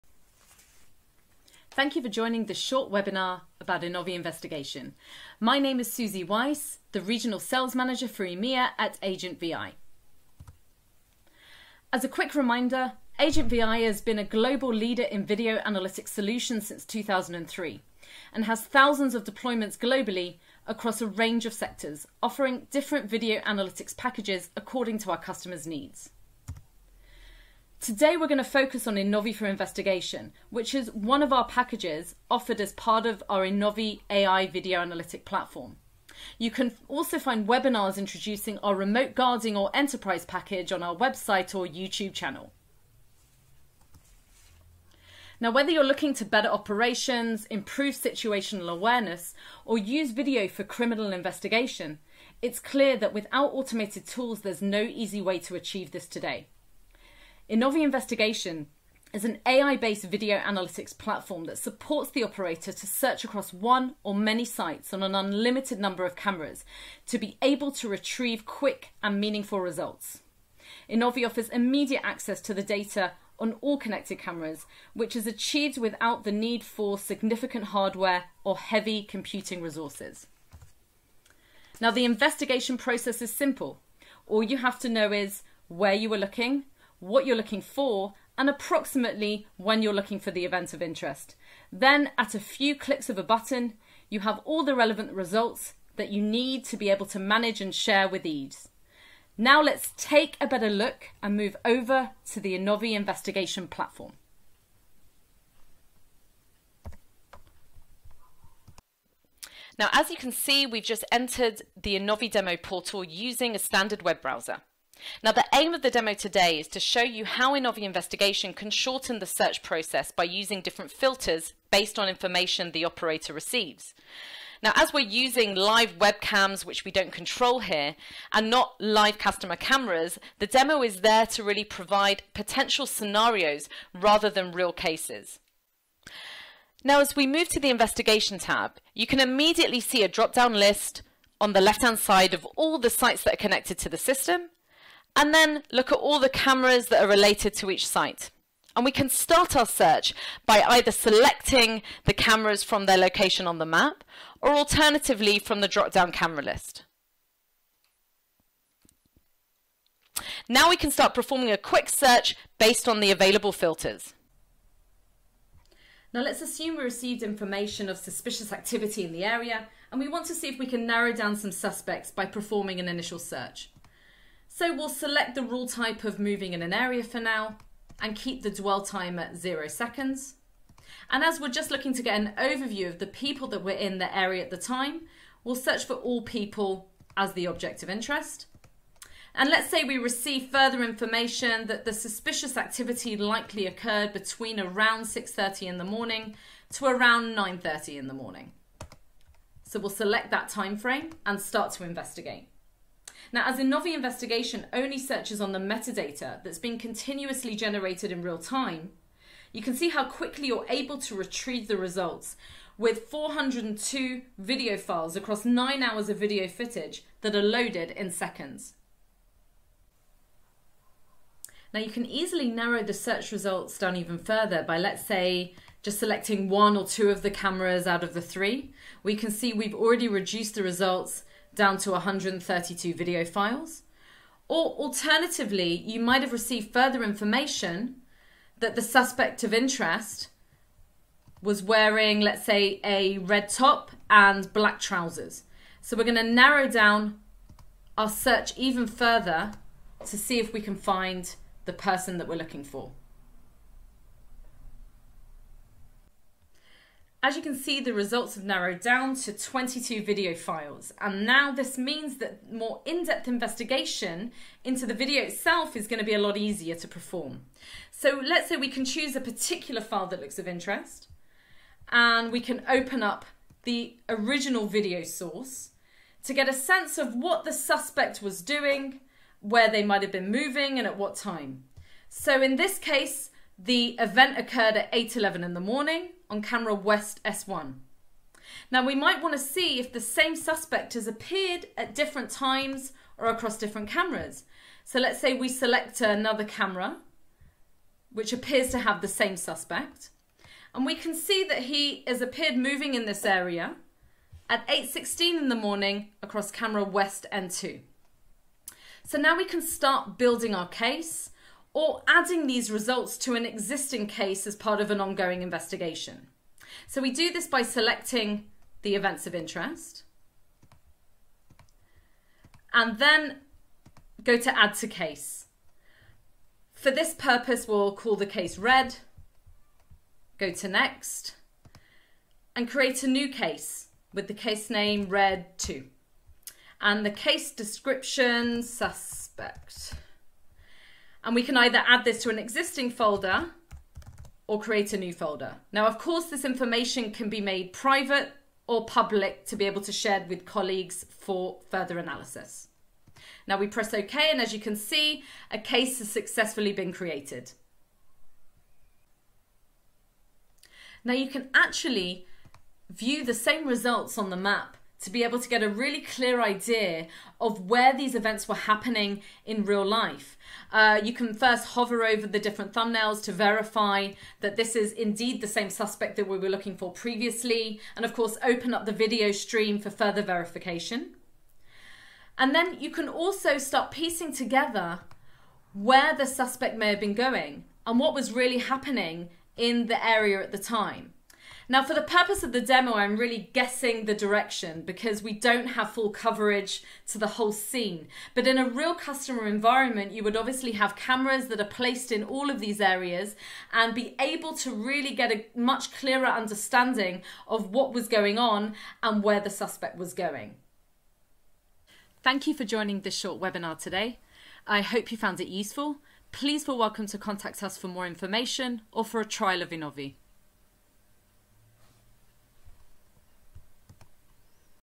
innoVi Investigation Recorded Webinar
Watch this 7 minute recorded webinar to learn about our AI- Powered Video Analytics product- innoVi Investigation.